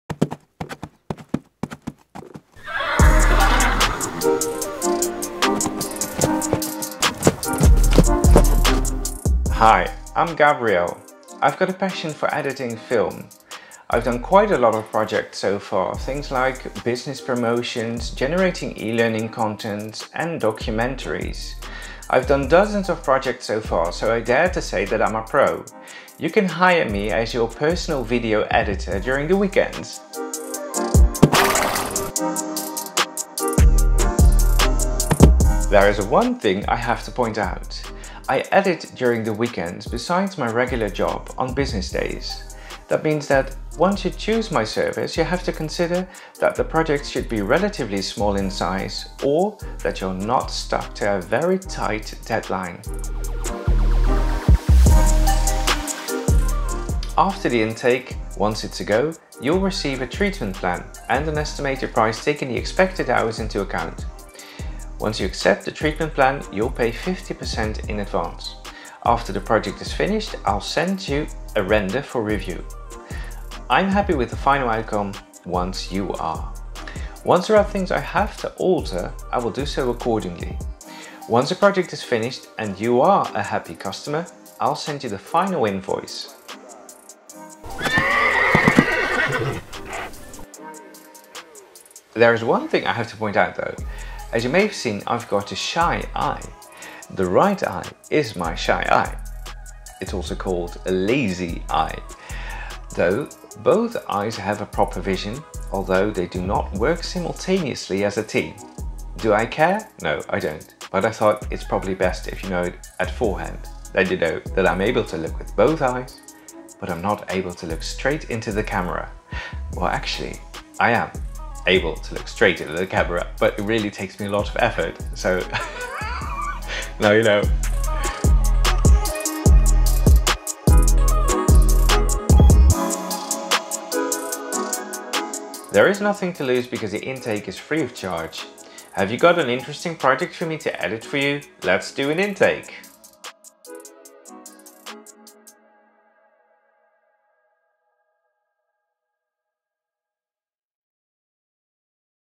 Mijnfilmstart nl - Hire me as your personal video editor
A promotional teaser about the video editing services provided by Gabriel Kortenoeven